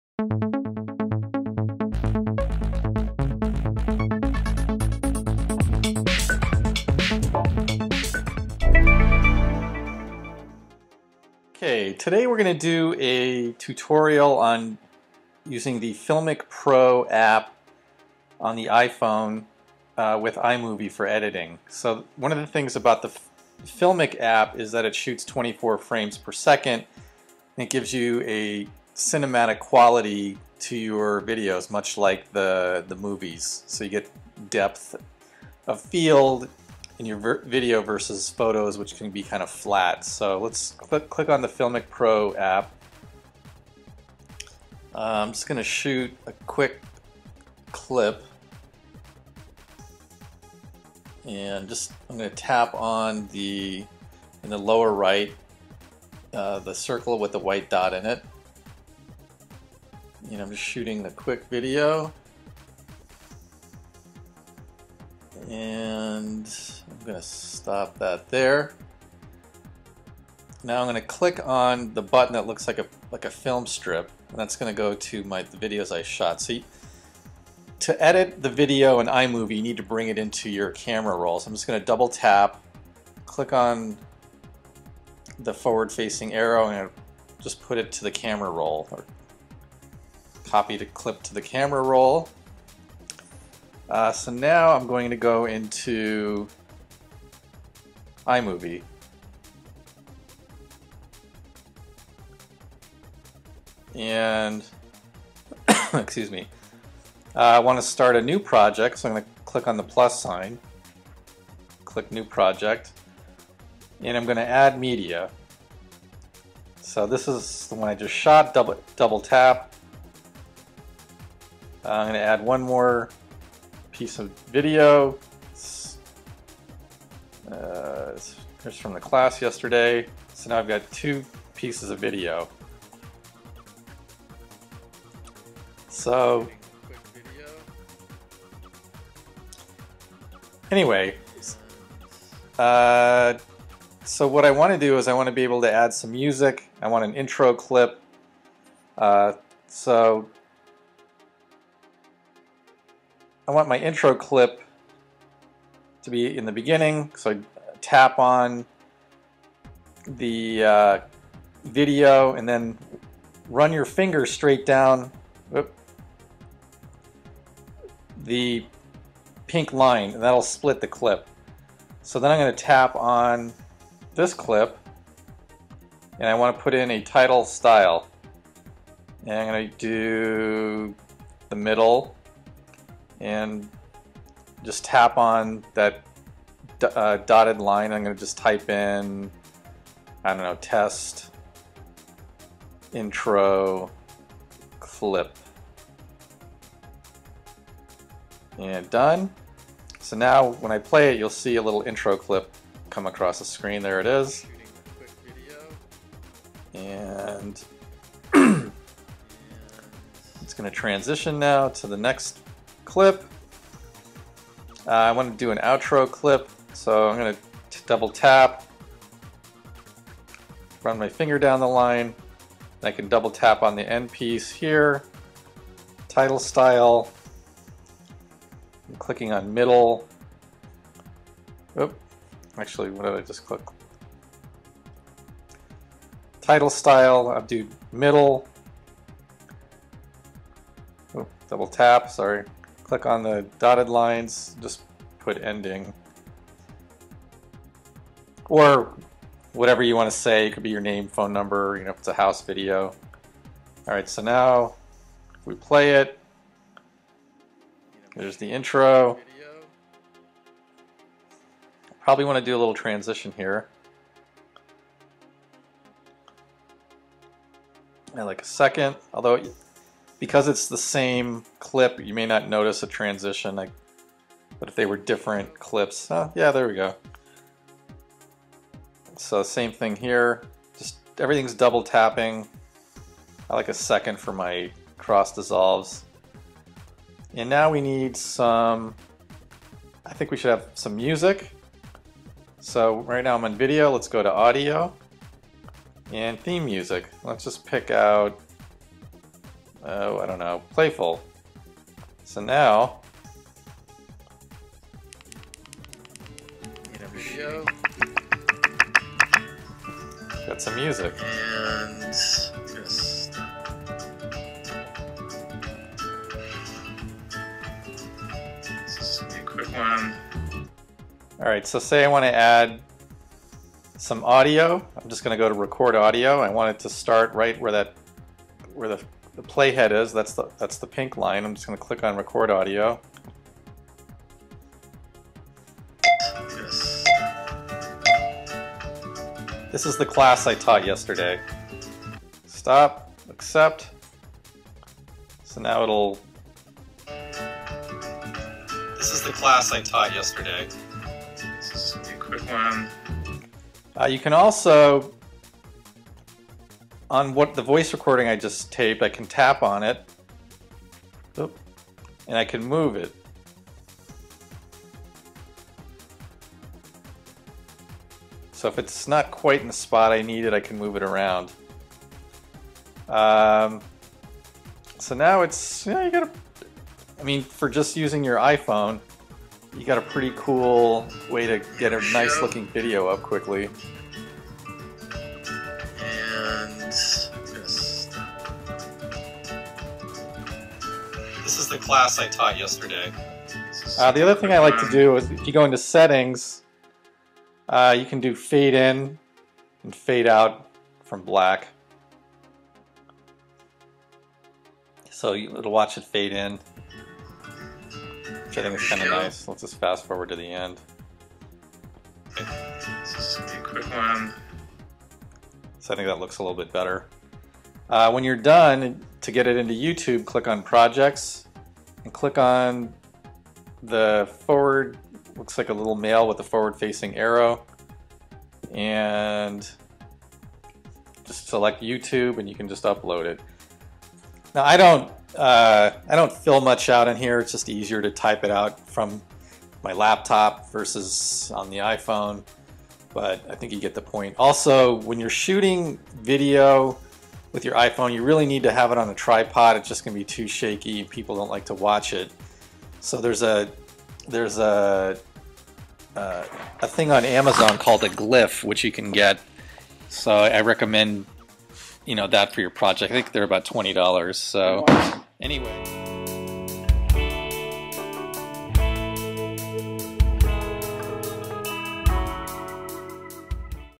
How to edit video on the iPhone with FilmicPro App
This video will demonstrate how to shoot video using the filmicpro app. The native iPhone app makes it hard to lock focus and exposure. With FilmicPro you can do both and keep your images constant all the way through the clip you are shooting. No more focus jumping.